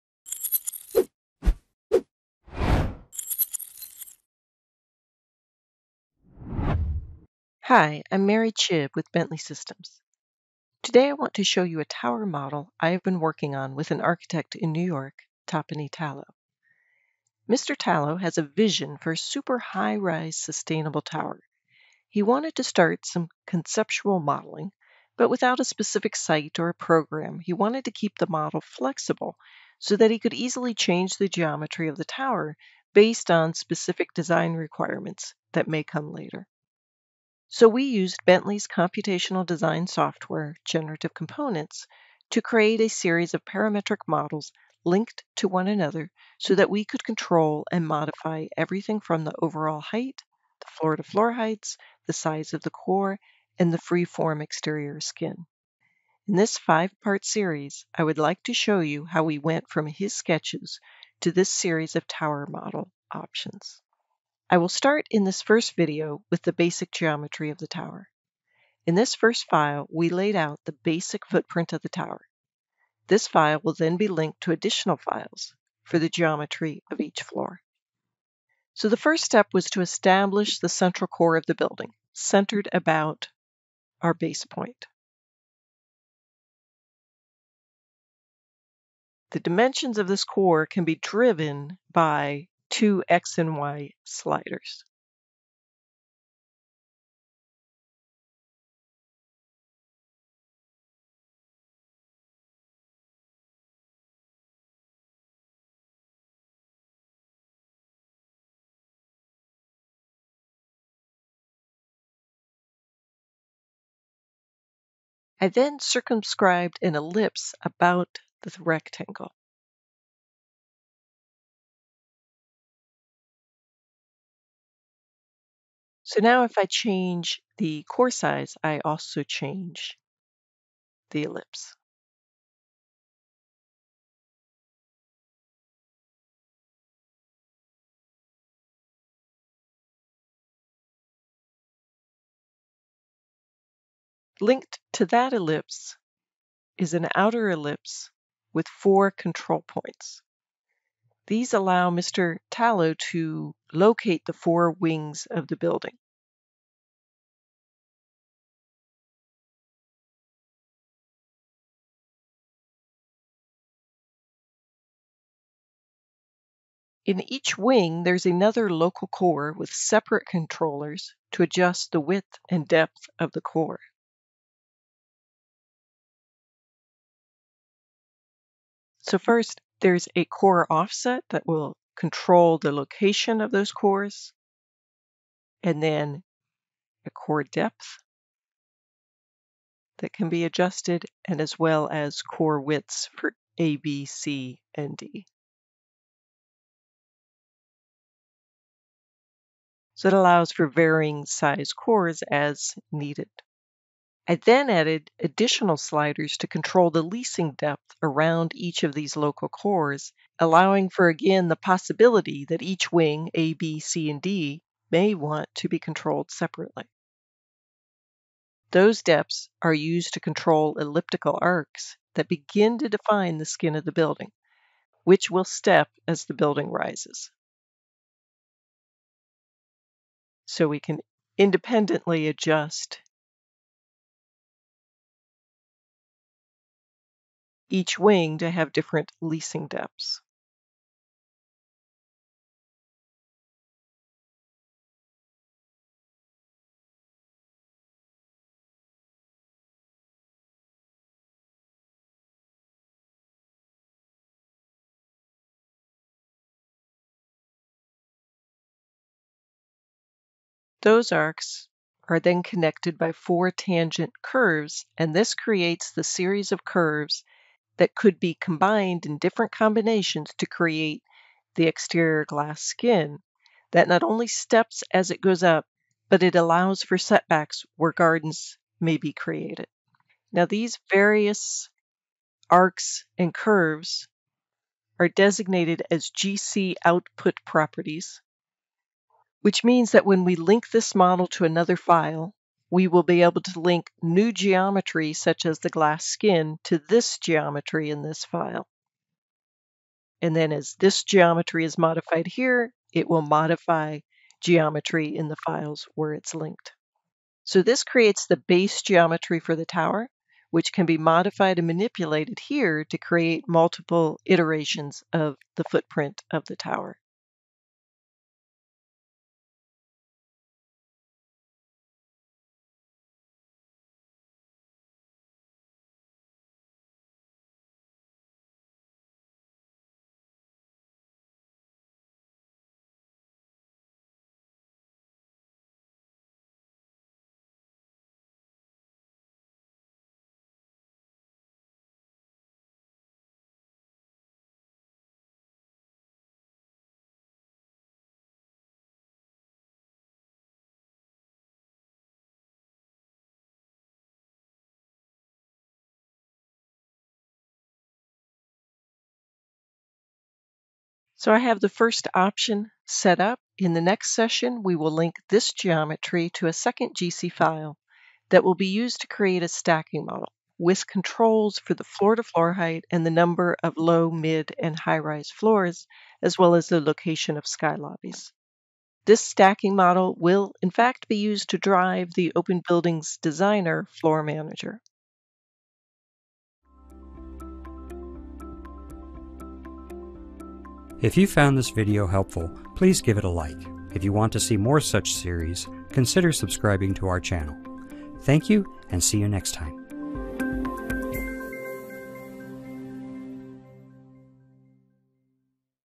Designing a Skyscraper using GenerativeComponents: 01 Establishing the Tower Geometry
Using an incredible design vision & hand-drawn sketches provided by architect Tapani Talo as the starting point, we show you how to use OpenBuildings GenerativeComponents to create parametric and associative geometry as the basis for an interesting high rise tower.
If you would like to learn more about Architect Tapani Talo and his thoughts on megatall buildings that are at the center of his design refer to his website and blog posts.